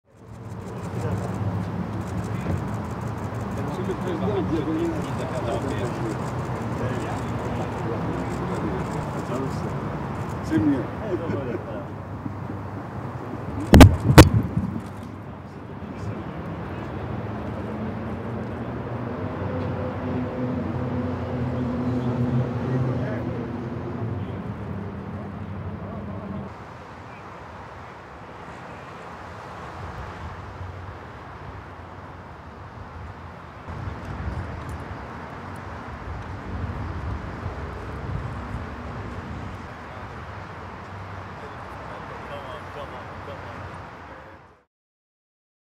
France's Macron arrives at EU MED summit in Athens | AFP
French President Emmanuel Macron arrives for a summit of southern EU leaders that is set to focus on climate change and "security challenges" including migration and the Afghan crisis.